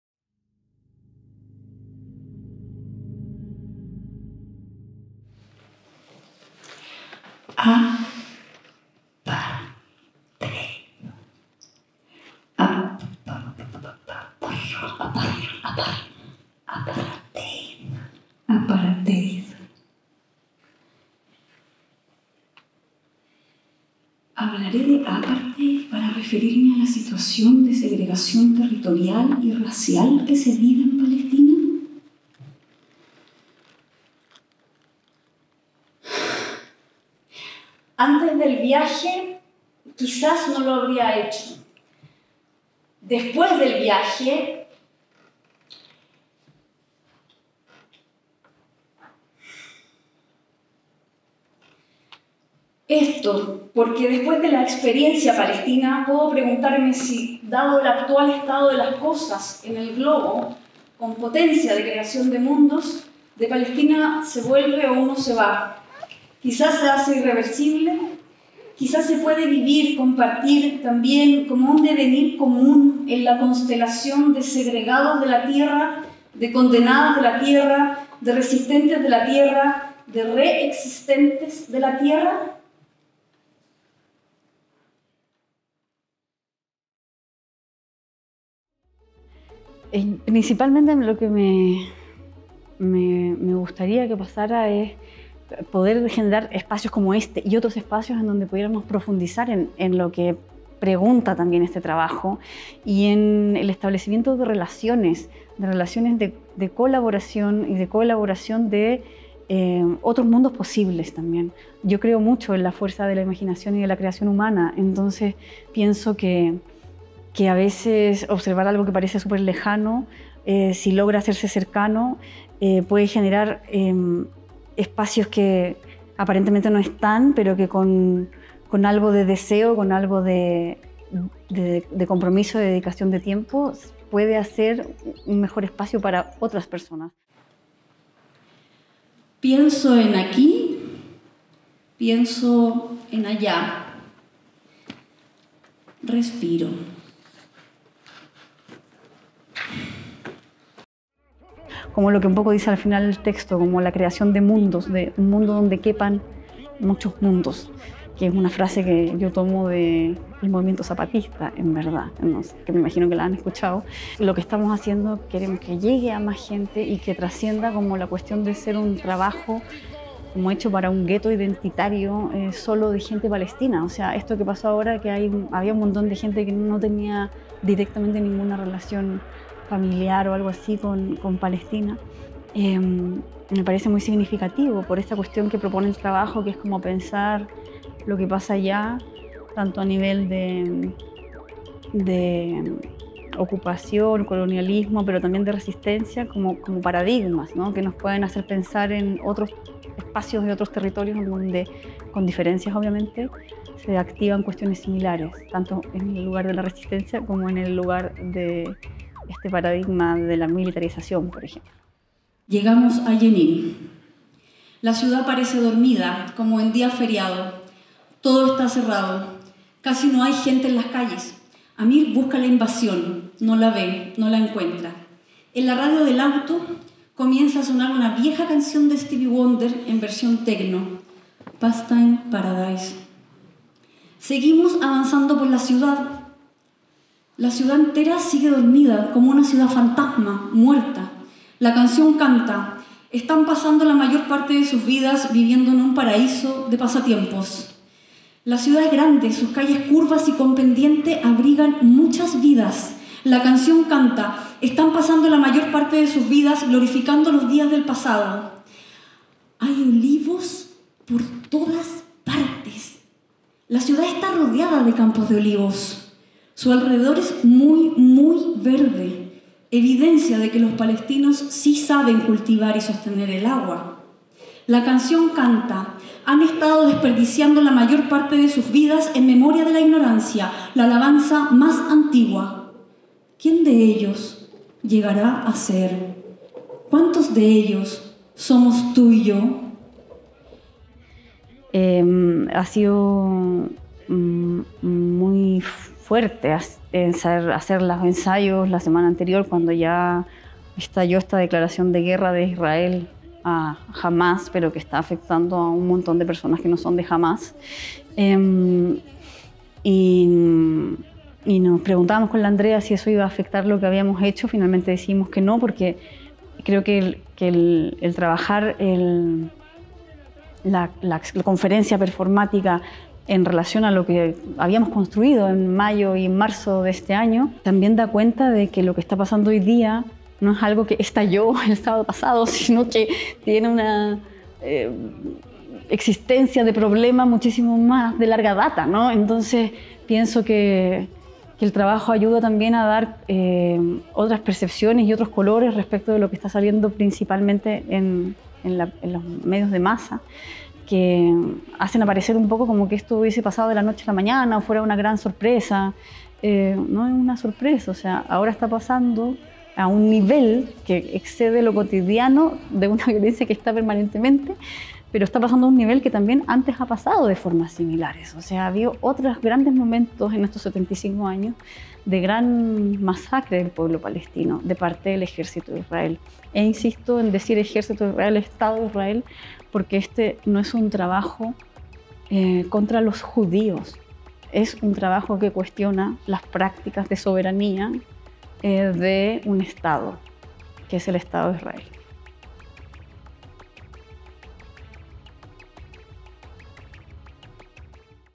Palestina irreversible. Palestina in-existente: (TNCH - Foro de las Artes 2023)
La conferencia-pieza-paisaje “Palestina irreversible. Palestina in-existente. Sonidos para la resistencia y la re-existencia”, —de Ana Harcha, Directora de Creación de la Facultad de Artes, en conjunto con la artista Andrea Giadach— es una exploración creativa que se presentó el 16 de octubre de 2023 en el Teatro Nacional Chileno, como parte de la programación del Foro de las Artes 2023: "Memorar al Porvenir".

Situada en un espacio entre la memoria y la ficción de un viaje a Palestina, esta puesta en escena se presenta como una experiencia que, través de imágenes, palabras y sonidos, invita al público a reflexionar sobre temas como la diáspora, el desplazamiento, la identidad y la creación artística, al tiempo que promueve la exploración de lo “salvaje” como una fuente de poder creativo.